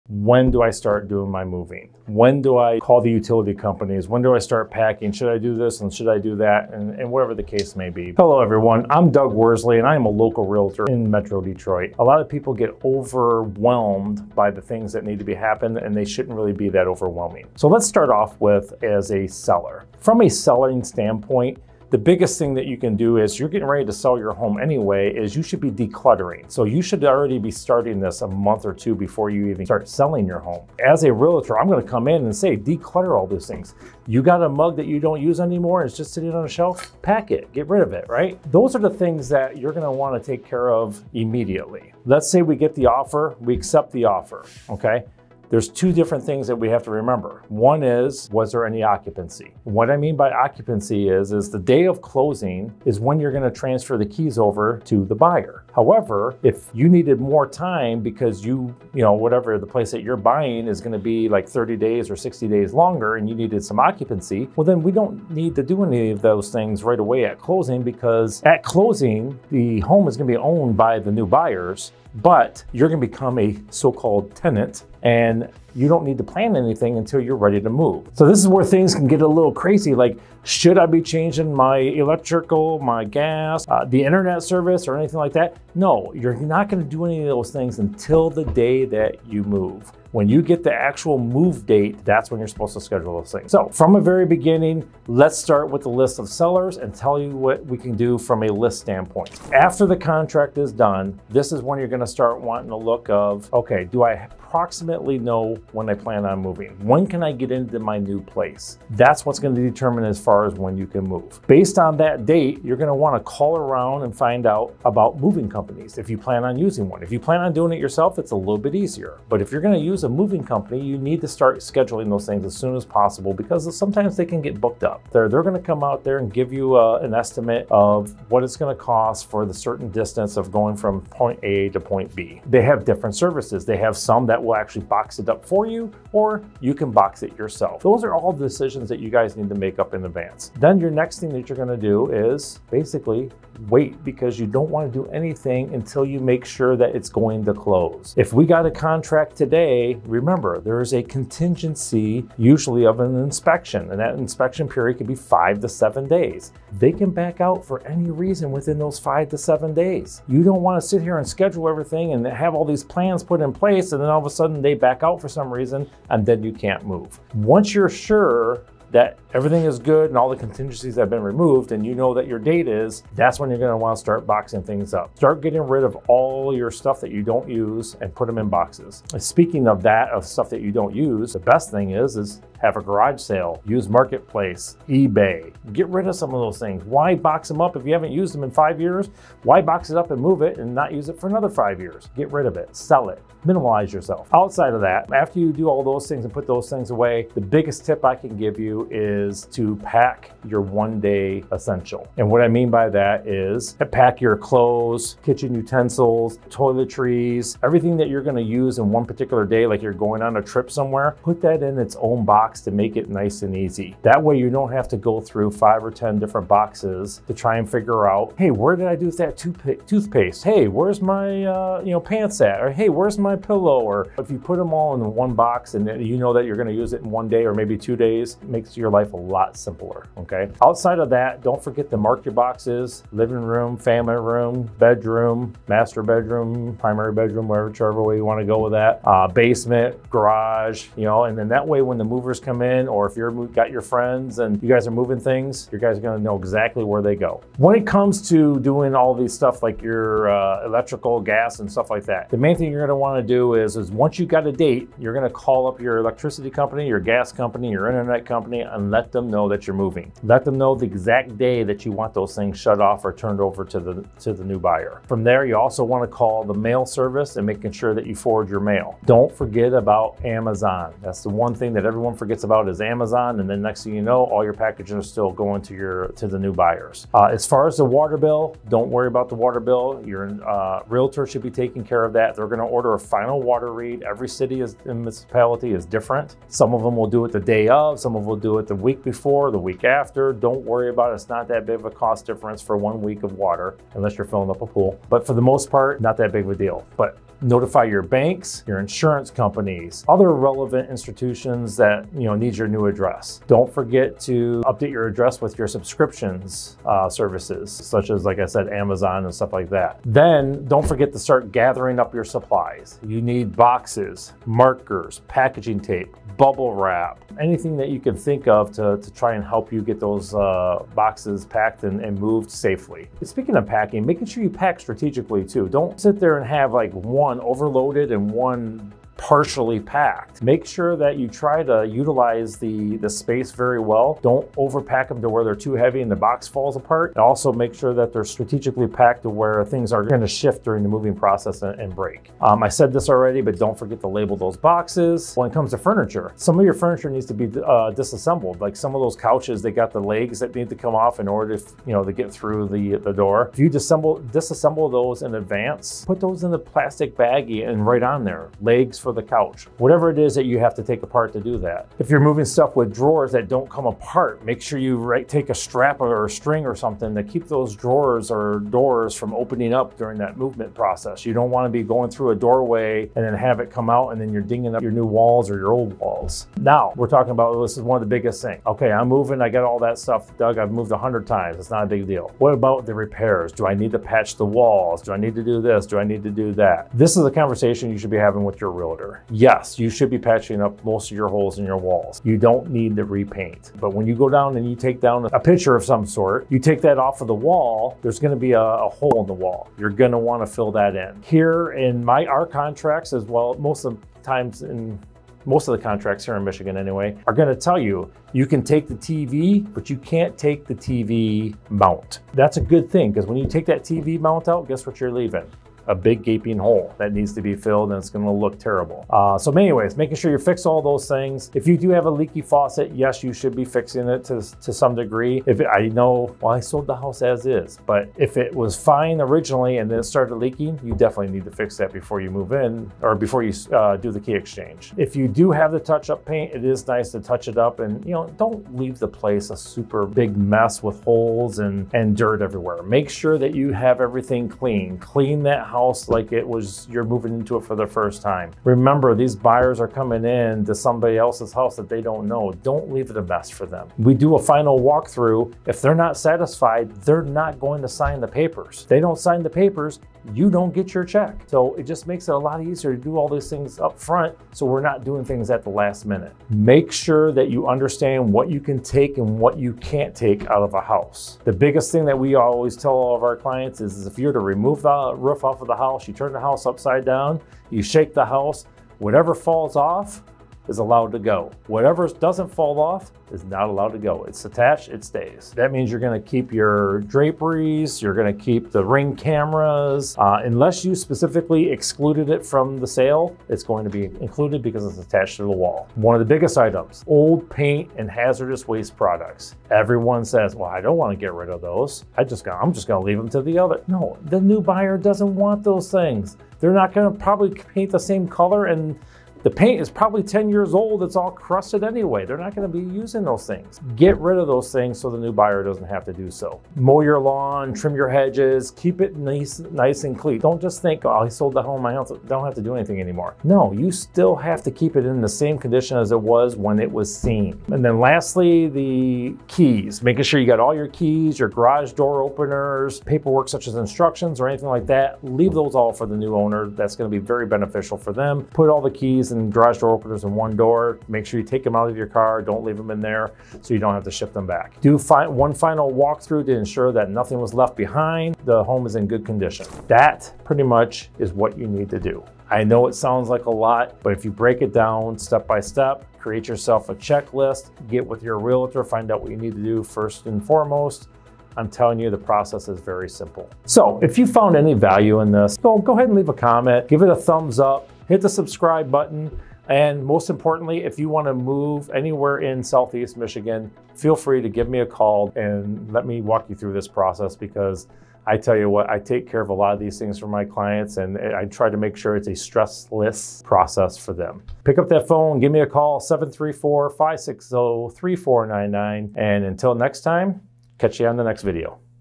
What Updates Should I Do BEFORE I Sell?
In this video, I go over some of the best things that you should do as a SELLER before you move out of your home. Whenever you are selling your home and also buying a new one, there are always some questions that arise that your Realtor should be able to help you with!